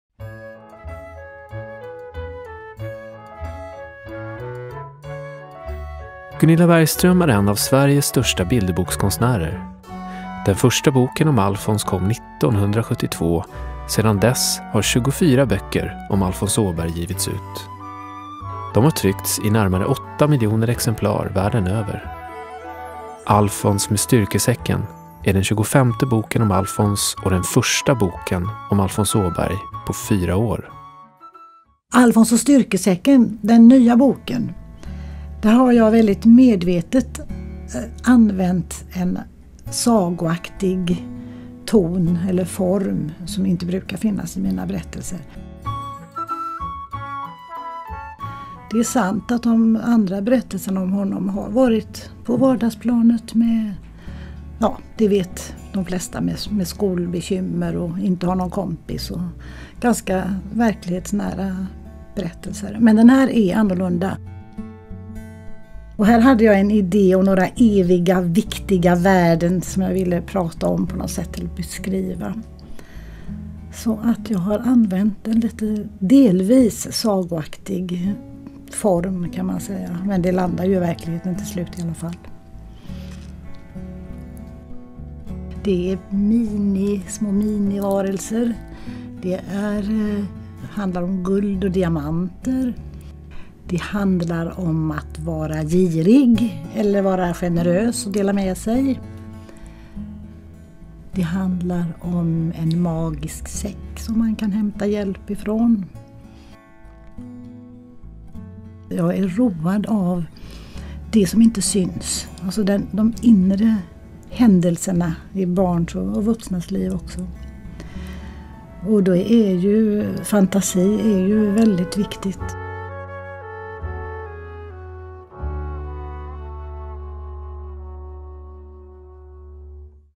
Gunilla Bergström berättar om nya Alfons
Gunilla Bergströms berättelser brukar kännetecknas av realism. Men här kommer för första gången en Alfonsberättelse i sagans värld!